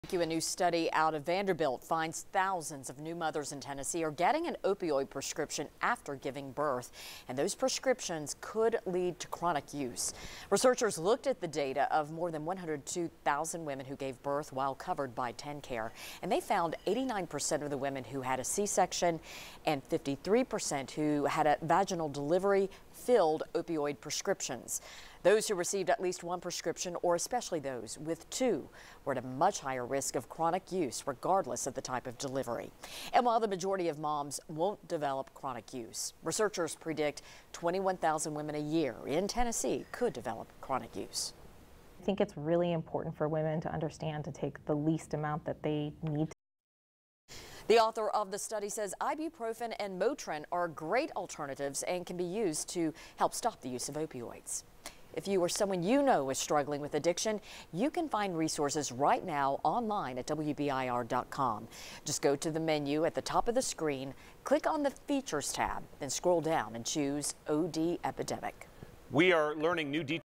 Study outlines postpartum opioid concerns
A new study out of Vanderbilt finds thousands of new mothers in Tennessee are getting an opioid prescription after giving birth.